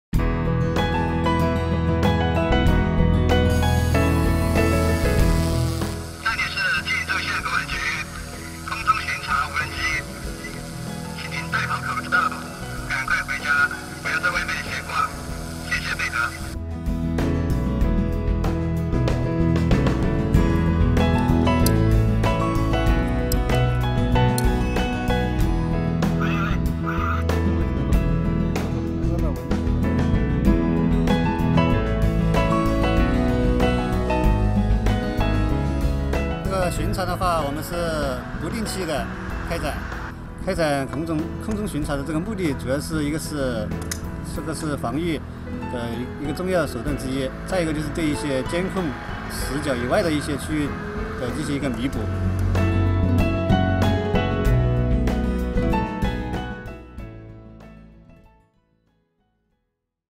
Coronavirus control: Drones with thermal imaging technology adopted in central China
Drones have joined fight against novel coronavirus (COVID-19) in China. Equipped with thermal imaging technology, they can detect those with a fever up in the air. Check out drone patrol in central China's Hunan